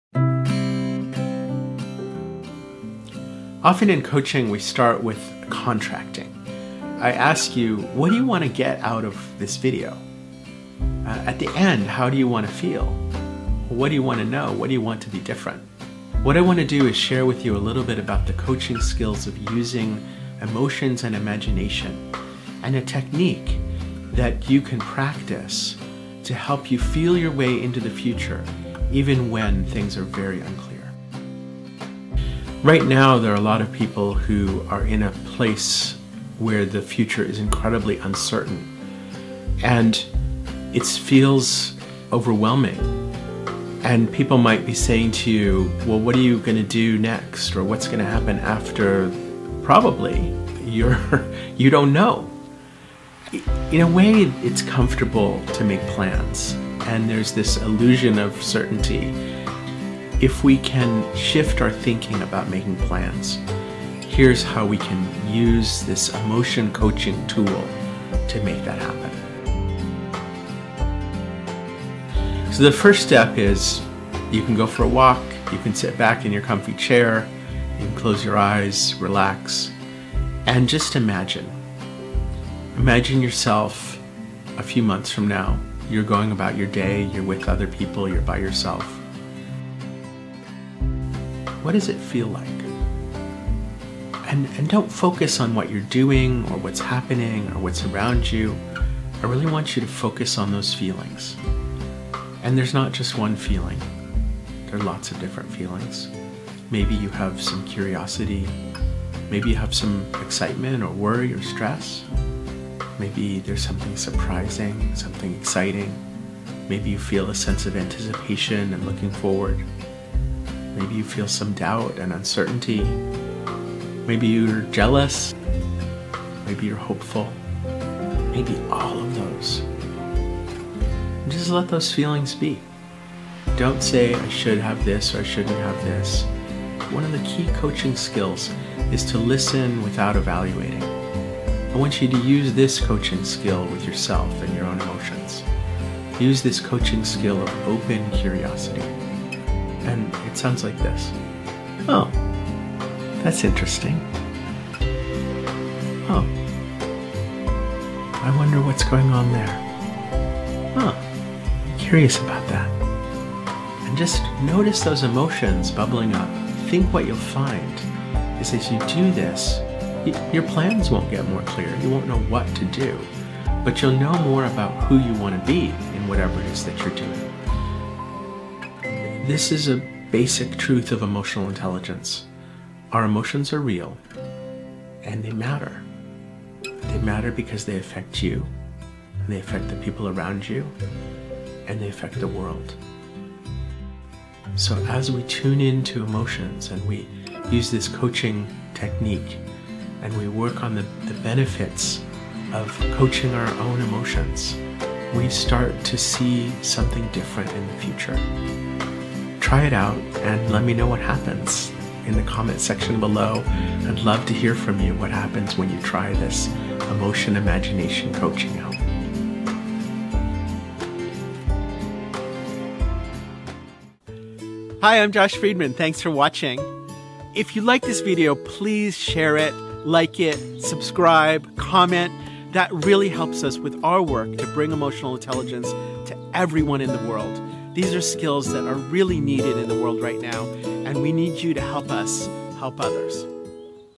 Self coaching tools | Stuck? Tune into your emotions & move forward
How do you want to feel? Get more self coaching skills using emotion and imagination including a self coaching technique that you can practice.  Learn from ICF Master Certified Coach Joshua Freedman MCC from Six Seconds Network as he teaches how emotion and imagination can help you feel your way into the future. Learn more about emotional intelligence, how to use self coaching skills like emotions to create a positive future.

Emotional Intelligence at Work | Master Your Emotions